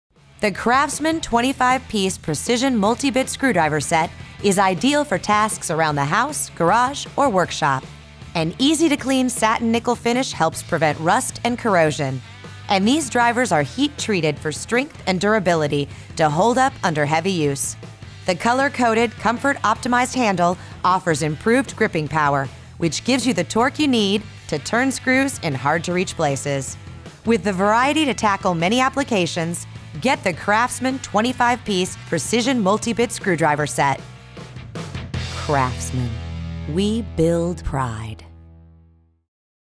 CRAFTSMAN 25 Piece Precision Multi-Bit Screwdriver Set | Tool Overview
Never be without the right screwdriver. The 25 Piece Precision Multi-Bit Screwdriver Set has the screwdriver you need for any job. Alloy-steel blades are heat-treated for strength and durability. (SKU:CMHT65046 )

Tool specs:
• Coated with a satin-nickel finish
• Comfort optimized handle for high & low torque applications